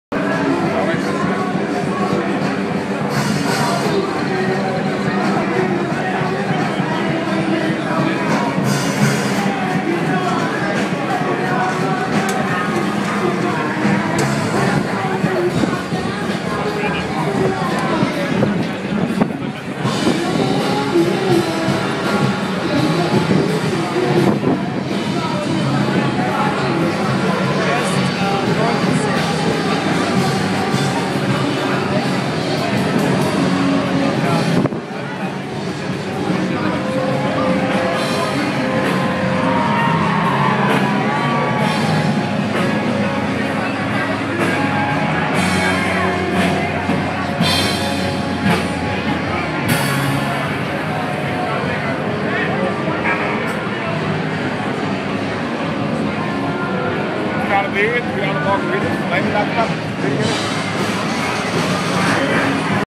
South by Southwest (SXSW) 2011: Walking Along 6th Street on March 16
This was my first year in Austin, Texas, for the South by Southwest festival, so this was my first time witnessing the sheer life and bustle of 6th Street as the music conference got into full swing. I tried to capture some of that here in this short video. (Sorry for the shakiness; I was walking when I captured this video on my iPhone.)